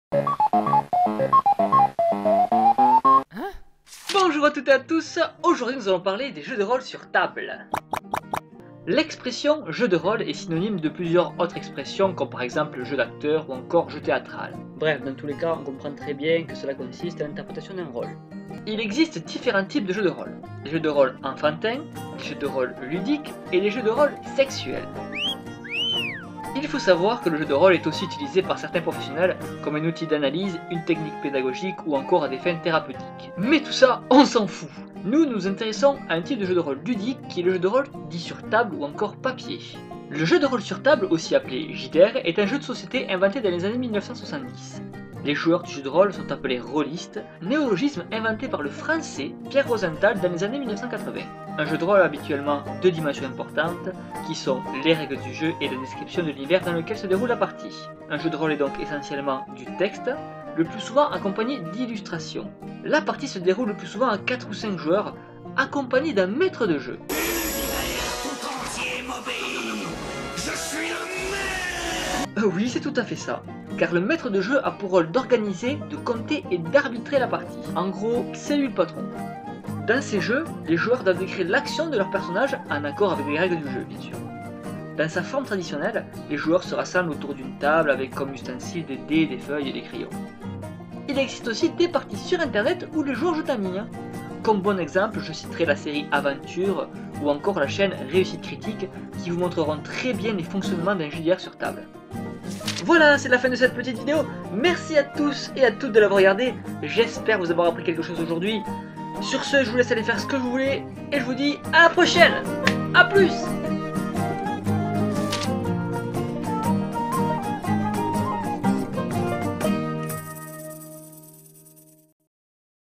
Les jeux de rôles sur table
Petite vidéo sur un genre de jeux de société peu connu mais qui mérite de l'être plus!
Pour résumé très brièvement cette vidéo: "Qu'est ce qu'un jeu de rôle?"
Eh bien c'est une histoire raconté par un type (le maître de jeu) dont les acteurs sont les joueurs!

N'hésitez pas à commenter et à me proposer d'autres sujets! ;)